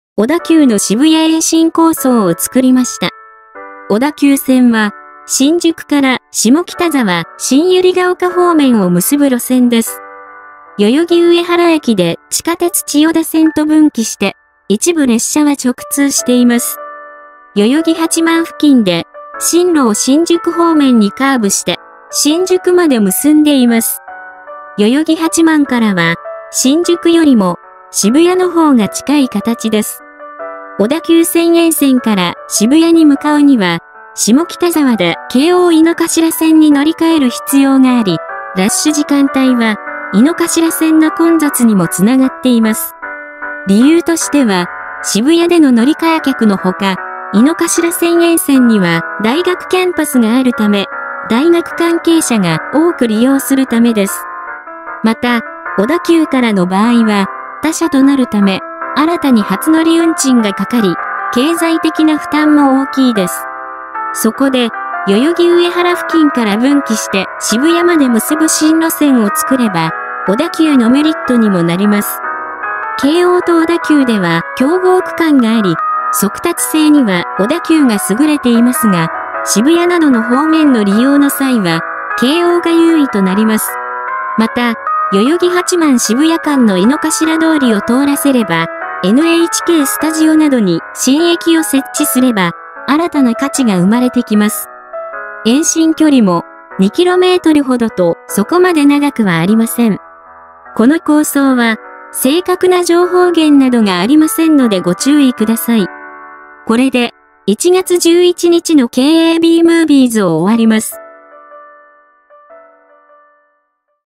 【小田急が渋谷に延伸？】『小田急渋谷線』の新路線構想は現実的なのか（2024年1月11日のニュース）
▼関連動画▼

【作品No.】
※問い合わせ時に必要となります。

※サムネ・動画内一部画像はGoogle Mapsより/画像加工/文字追加

※「構想」の説明についてはチャンネル概要欄をご確認ください。公式ソースはありません。